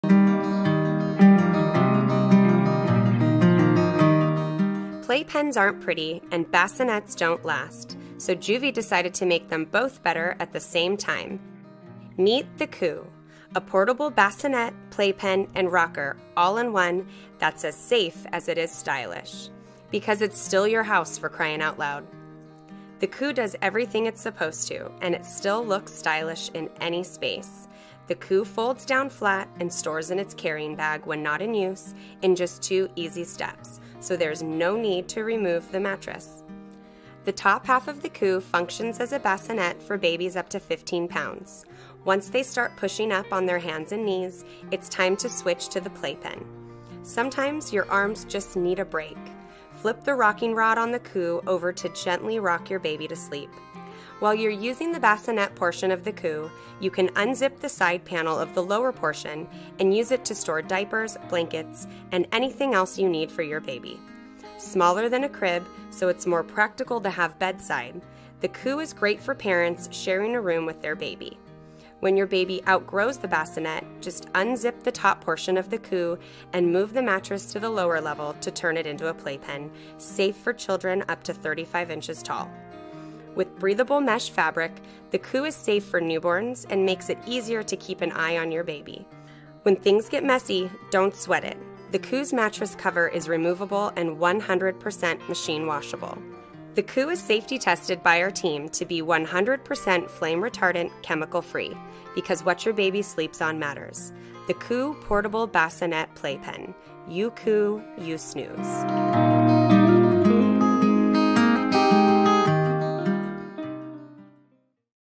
Coo Portable Bassinet Playpen, Gray Reviews
This is the best Playards in the marketplace with cheap price that you can easily afford.
Great for small spaces and perfect for room sharing families. Folds flat for travel and storage
Use it longer - goes from bassinet to playpen with a zipper. Convenient under-bassinet storage
Machine-washable mattress cover. Easy two-step fold. Comes with travel bag, mattress, and mattress cover
Rocks or stays put, you choose. 100% see-through and breathable
Color:Gray
Because it's still your house, for crying out loud. The Coo does everything it's supposed to, and still looks stylish in any space. The top half of the Coo functions as a bassinet for babies up to 15 lbs. Once they start pushing up on their hands and knees, it's time to switch to the playpen. The premium breathable fiber mattress provides maximum comfort for your little ones. While you're using the bassinet portion of the Coo, you can unzip the side panel of the lower portion and use it to store diapers, blankets, and anything else you need for baby. When your baby outgrows the bassinet, just unzip the top portion of the Coo and move the mattress to the lower level to turn it into a playpen, safe for children up to 35"" tall (or until they can climb out). With breathable mesh fabric, the Coo is safe for newborns, and makes it easier to keep an eye on your baby. When things get messy, don't sweat it - the Coo's mattress cover is removable and 100% machine washable. Safety tested by our team to be 100% flame retardant chemical free - because what your baby sleeps on matters.
Coo Portable Bassinet Playpen, Gray